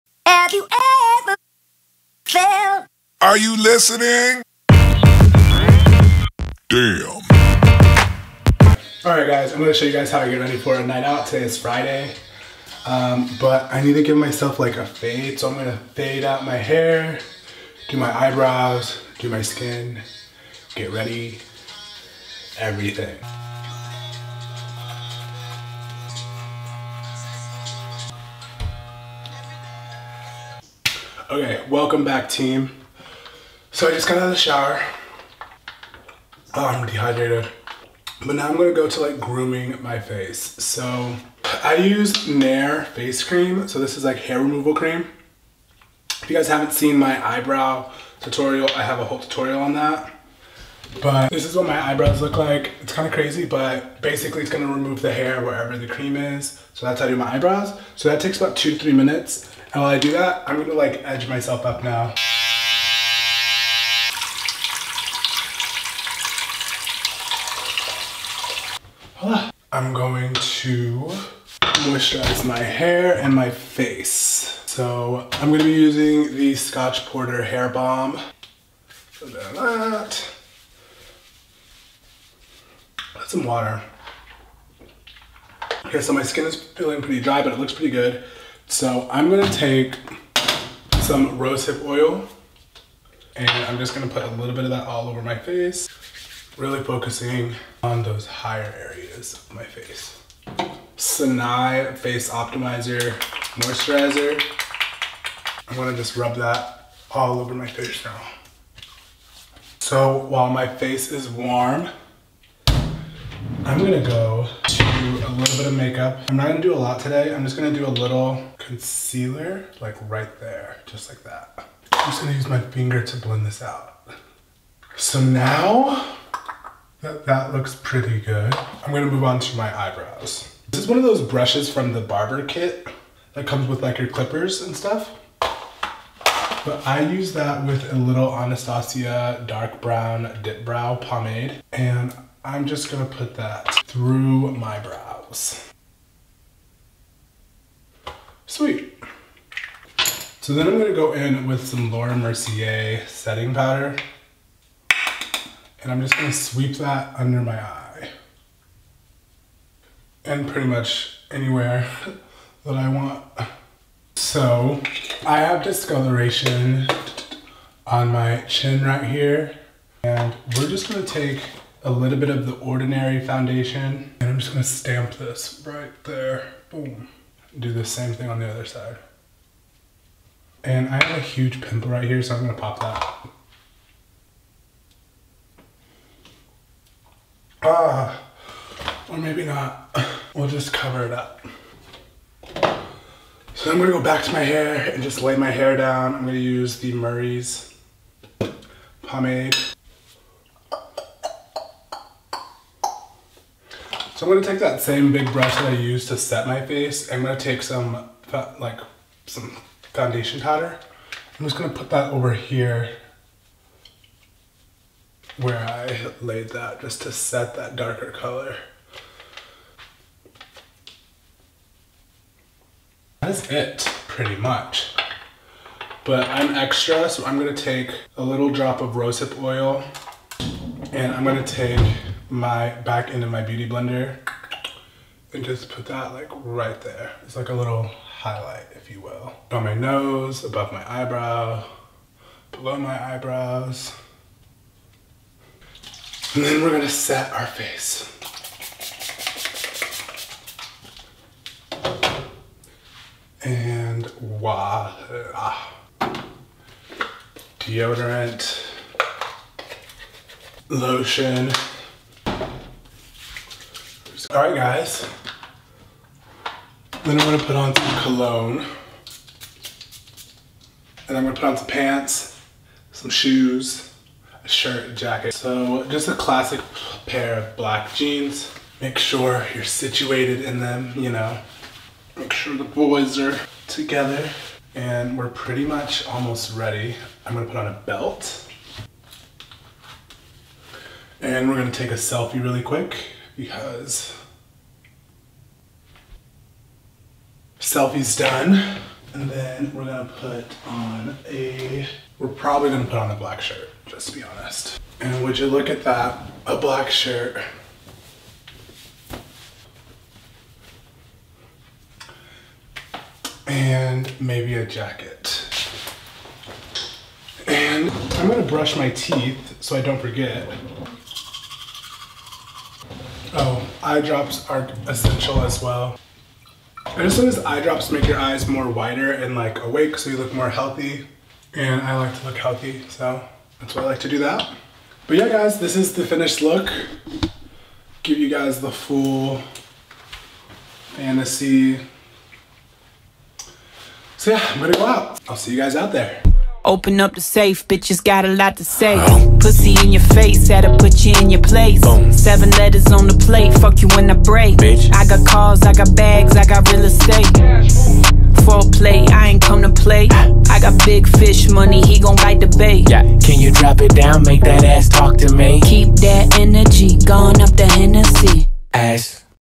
GRWM: LA NIGHT OUT Trell West’s Quick Men’s Look | Grooming Secrets
How the 24 year old get’s ready for a LA night out —  Inspired by Vogue's 'beauty' secrets.

Full product list below ↓

Face
Wahl t-blade trimmer
Nair Face cream
Rosehip Oil

Murray's Superior Hair Dressing Pomade
Thayer's Witch Hazel Facial Mist Toner